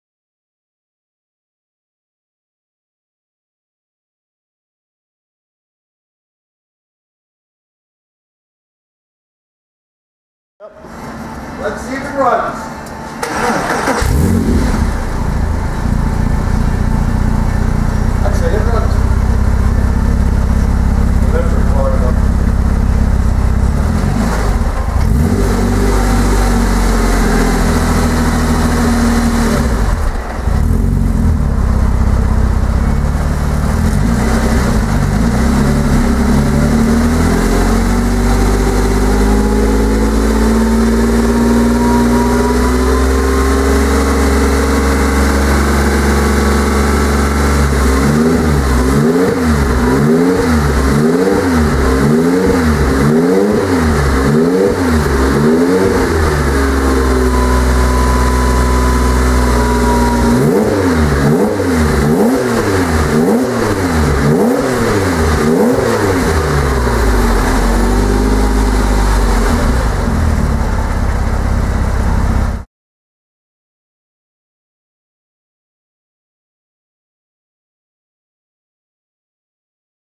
TPC Racing: 3.7L Cayman Turbo First Start
First start for a 3.7L Cayman Turbo built by TPC Racing.